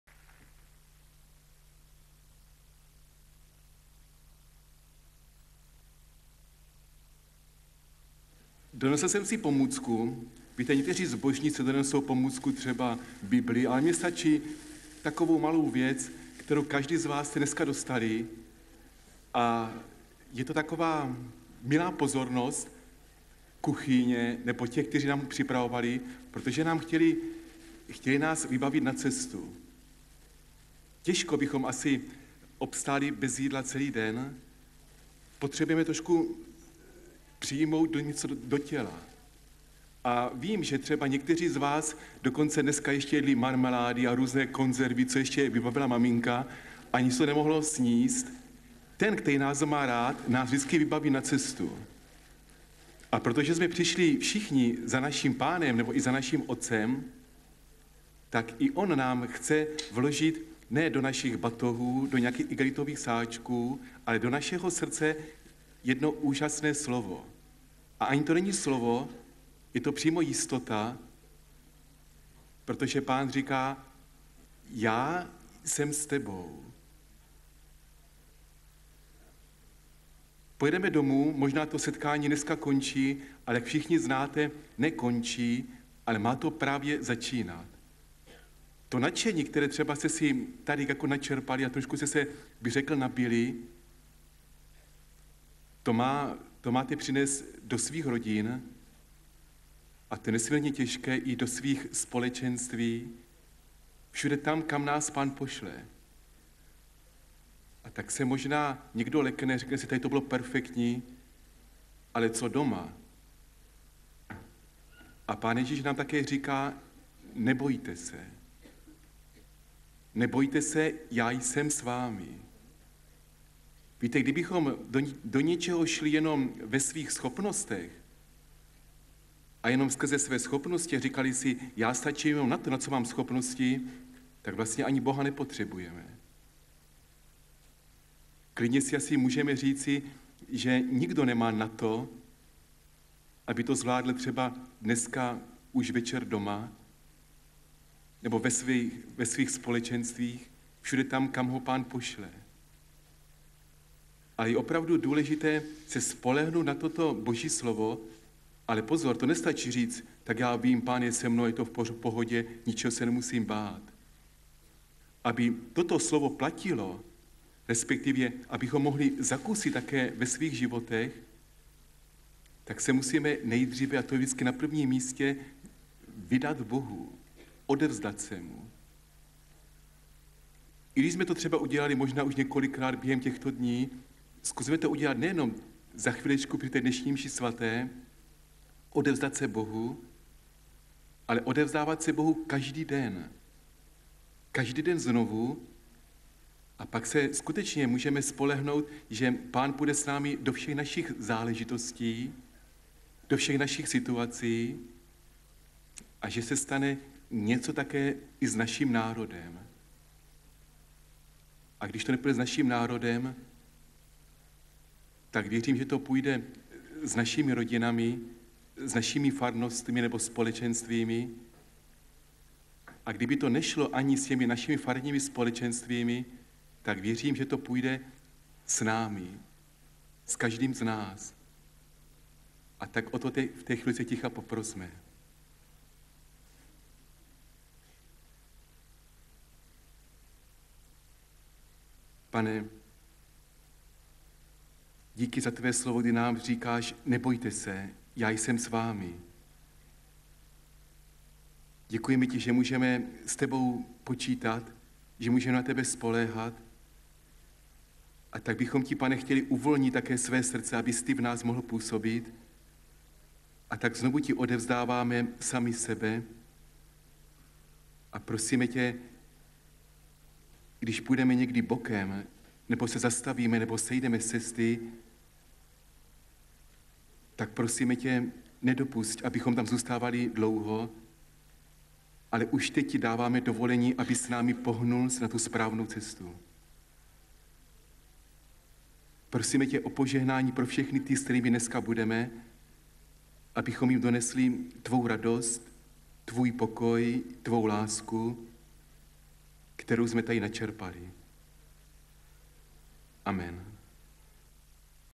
Svatá Hora ’99 — P. Ivan Fišar — slovo na den
Záznam slova na den P. Ivana Fišara na celostátním setkání mládeže na Svaté Hoře u Příbrami v roce 1999.